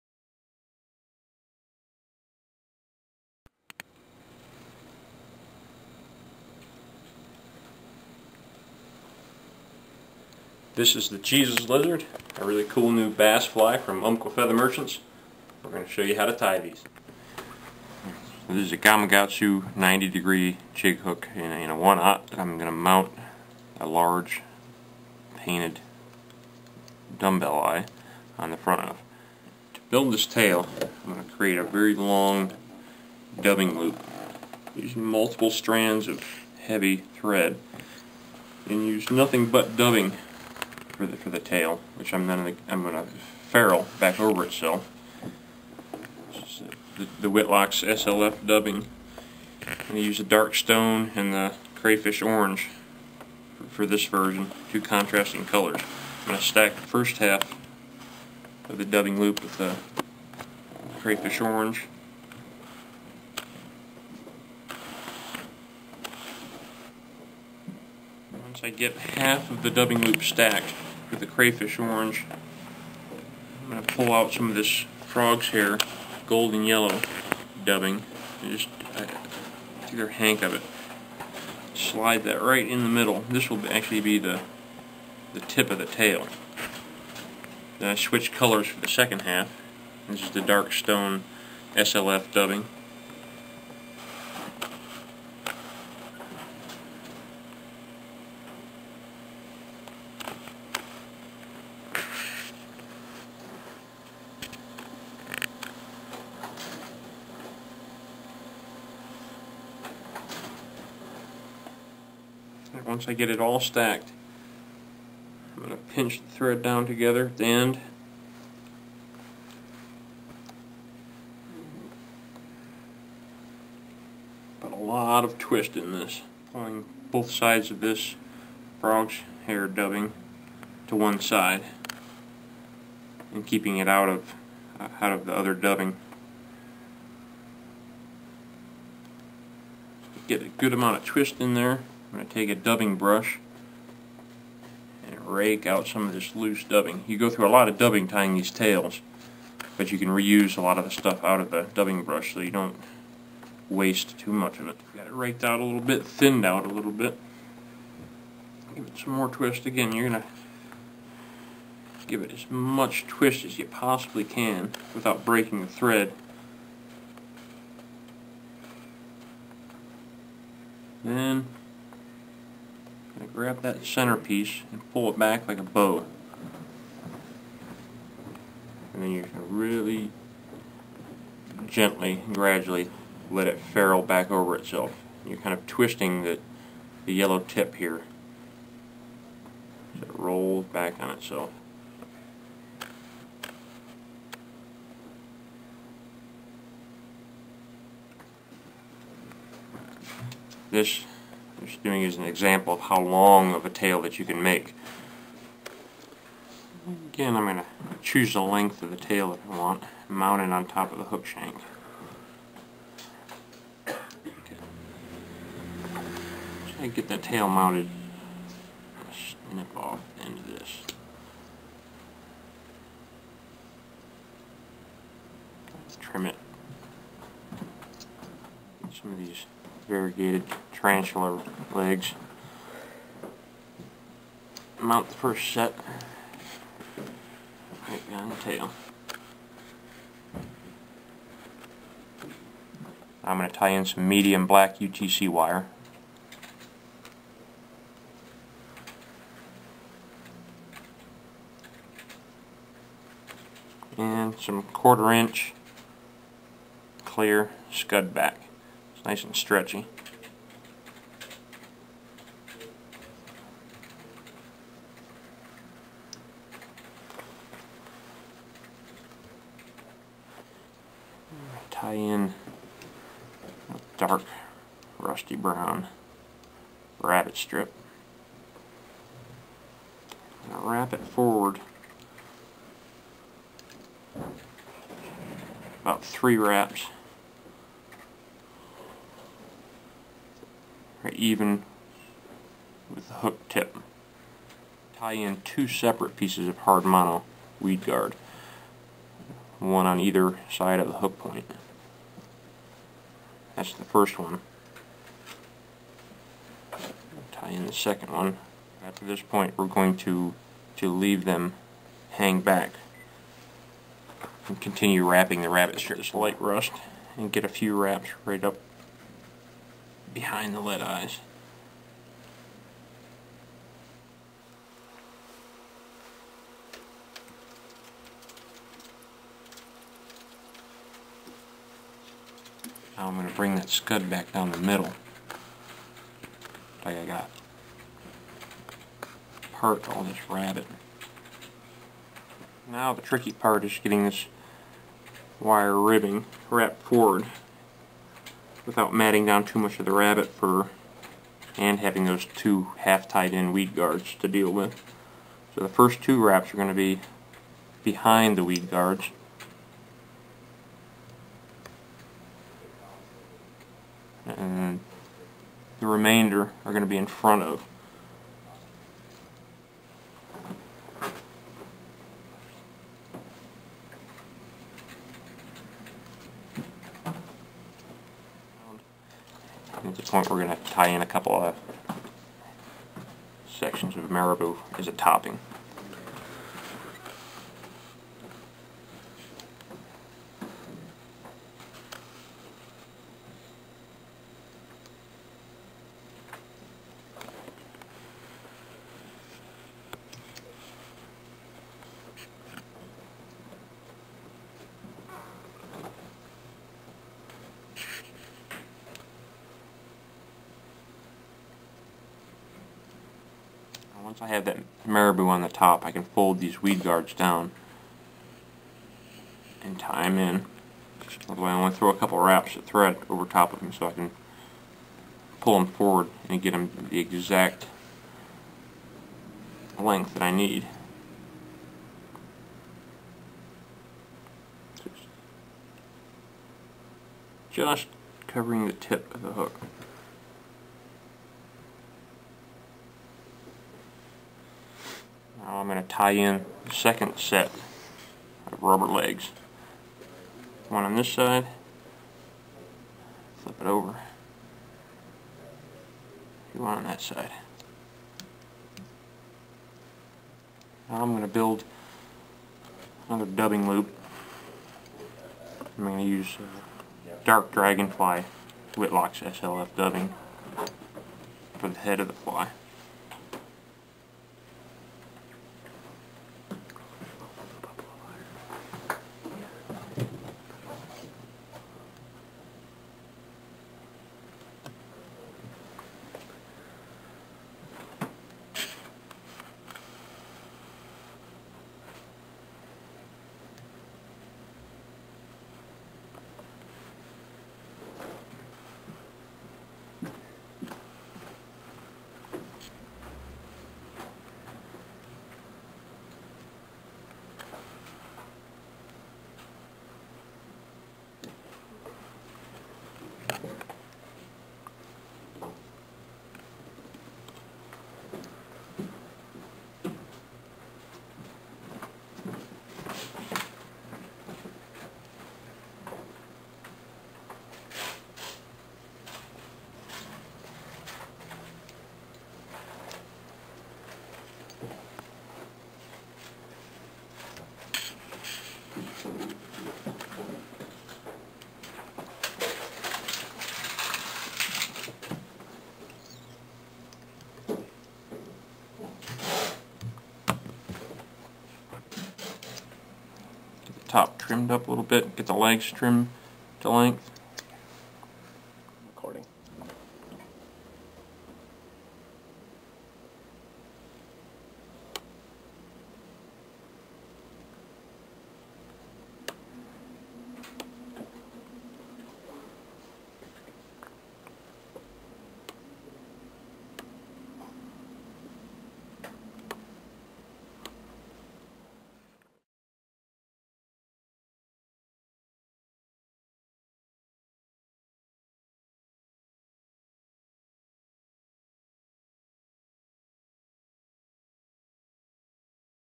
Geezus Lizard fly Tying Demo
Jay Zimmerman shows viewers his new Geezus Lizard that is now being tied by Umpqua feather for 2010.  Get your hands on a few before all the bass see them.